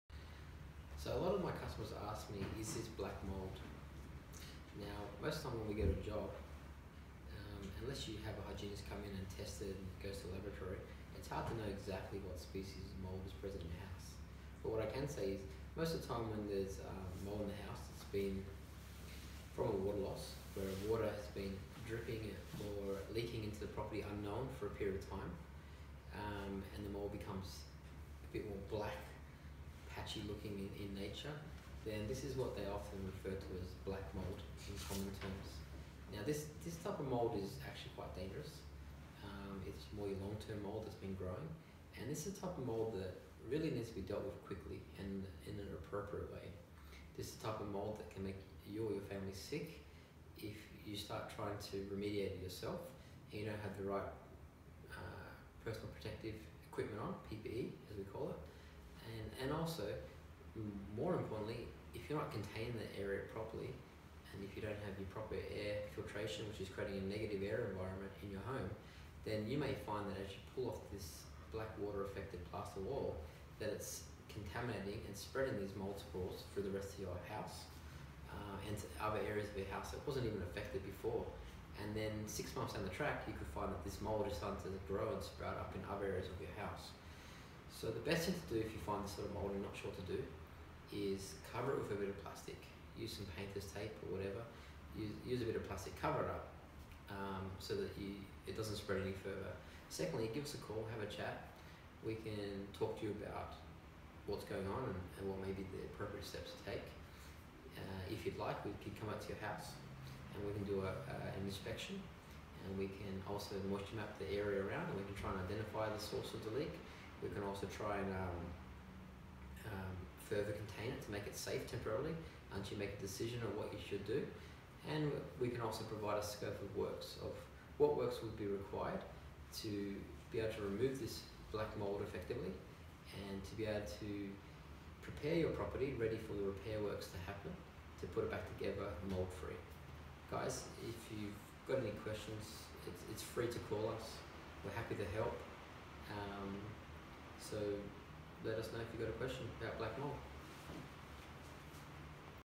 The difference between the 4 types of mould; black, blue, green & white.
Click here for our article on the four different types of mound, how to remove them and what they mean

What is the difference between black mould, blue mould, green mould and white mould?

There are four different types of mould. In this article, we explain the differences between black mould, blue mould, green mould, and white mould and outline the health risks with each type of mould